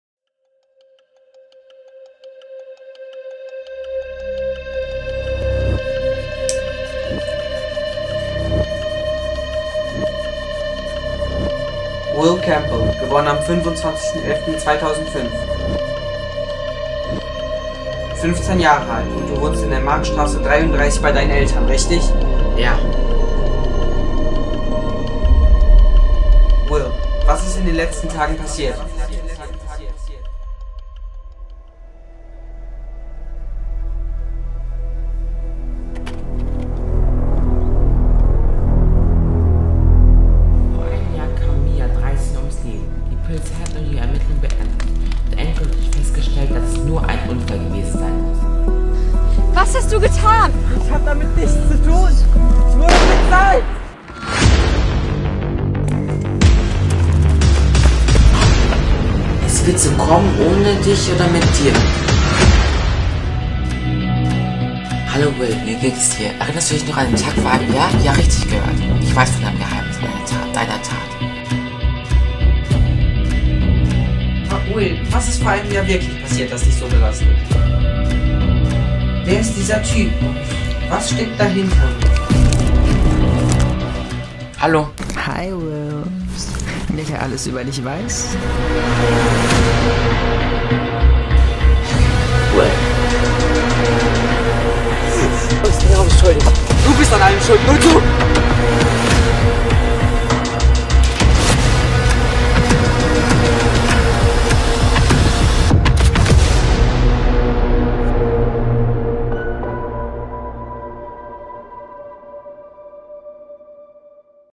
Trailer - CodeX Staffel 1
CodeX - BLACKSTORY STUDIOS ORIGINAL
November 2021

Der offizielle Trailer der vierteiligen Miniserie CodeX.
In diesem Monat ist es soweit und CodeX wird hier auf diesem Kanal released.
Vier kurze Folgen wöchentlich. Also seid jede Woche dabei, wenn es heißt "Neue Folge von CodeX".

Wie hat euch der Trailer gefallen? Seid ihr genauso gehyped wie wir?
Schreibt es in die Kommentare, denn Feedback ist wichtig und richtig.

@BLACKSTORY/STDUIOS